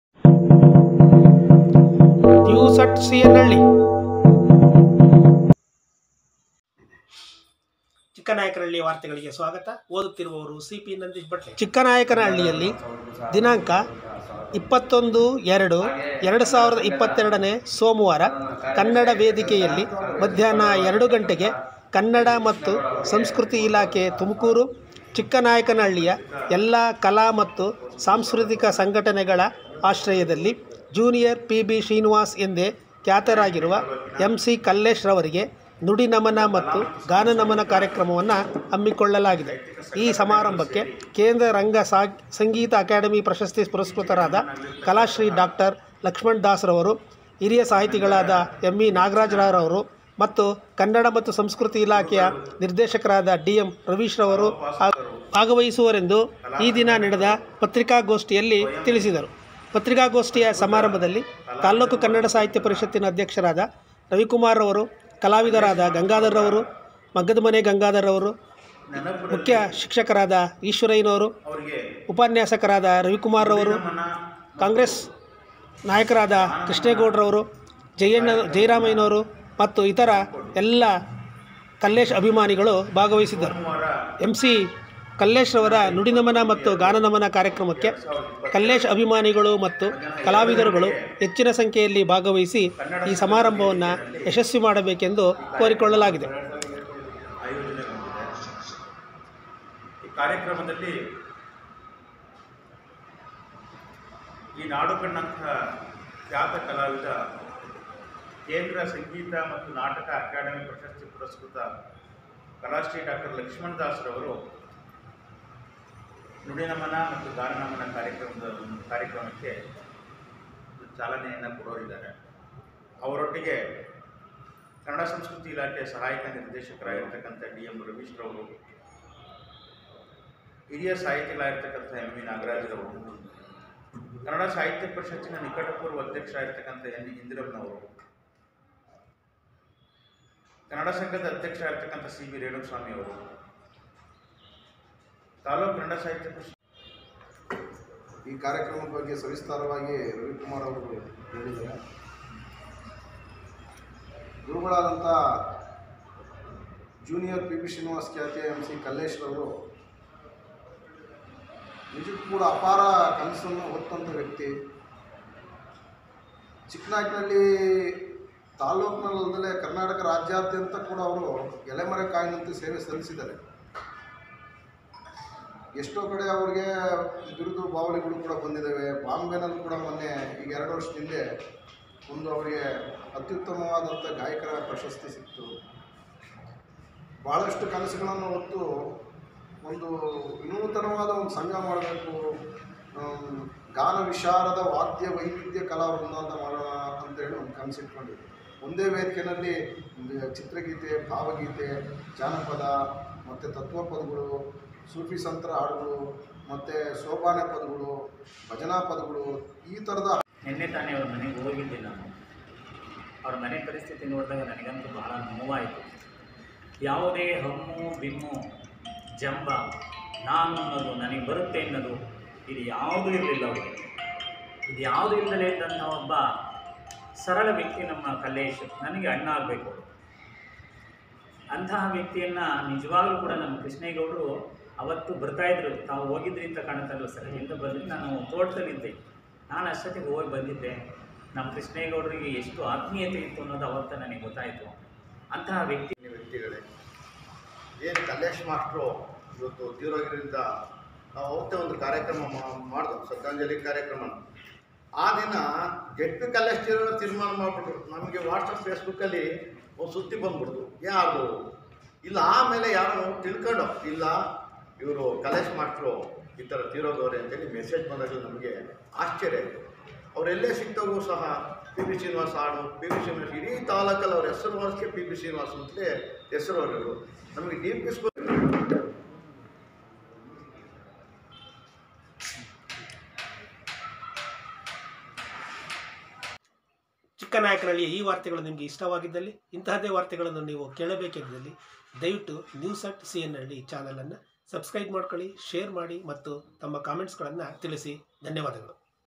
news@cnhalli:ಎಂ.ಸಿ ಕಲ್ಲೇಶ್ ರವರಿಗೆ ನುಡಿ ನಮನ ಮತ್ತು ಗಾನ ನಮನ ಕಾರ್ಯಕ್ರಮ, ಕನ್ನಡ ಸಂಘದ ವೇದಿಕೆ
news@cnhalli:ಎಂ.ಸಿ ಕಲ್ಲೇಶ್ ರವರಿಗೆ ನುಡಿ ನಮನ ಮತ್ತು ಗಾನ ನಮನ ಕಾರ್ಯಕ್ರಮ, ಕನ್ನಡ ಸಂಘದ ವೇದಿಕೆಯಲ್ಲಿ ದಿನಾಂಕ 21/02/2022